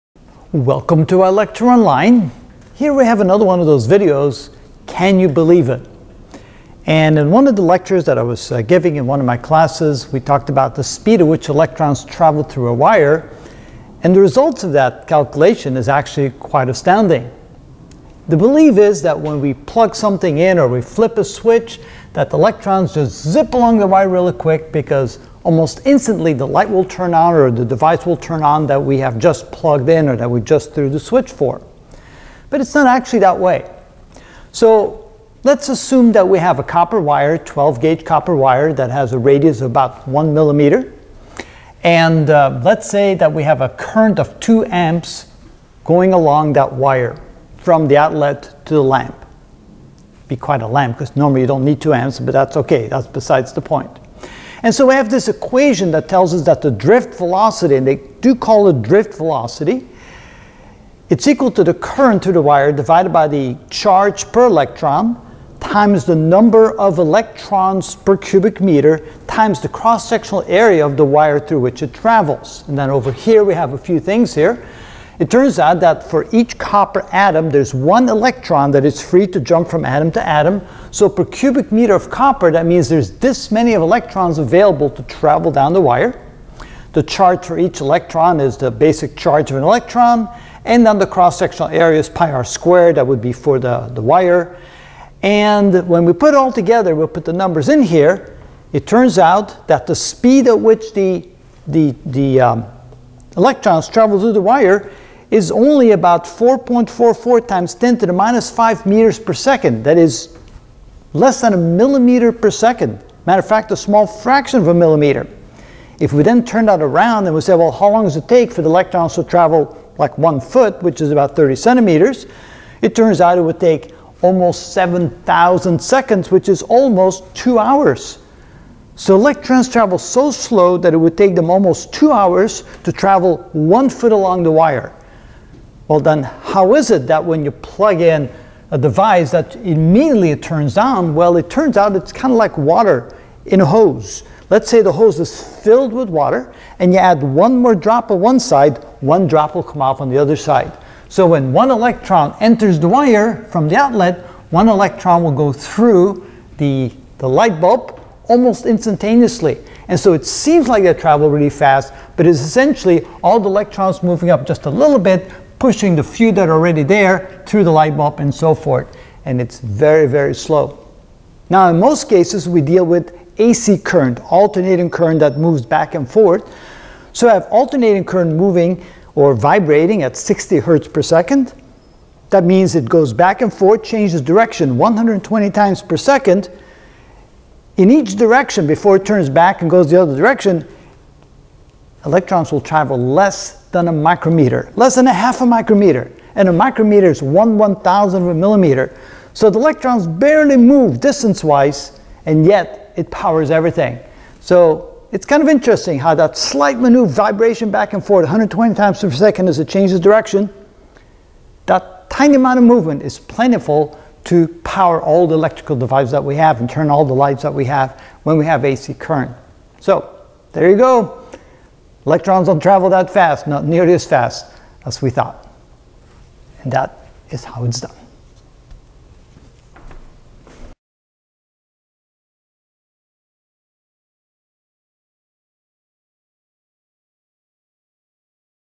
Can You Believe It? #64 How Fast Do Electrons Move In A Wire
We will calculate how fast electrons move in a wire.  You’re going to be surprised!

Title search on other platforms:  Can You Believe It? #63 The Incredible Edible Egg

Title search on other platforms:  Can You Believe It? #65 Why Are Transmission Lines Kept At Such High Voltage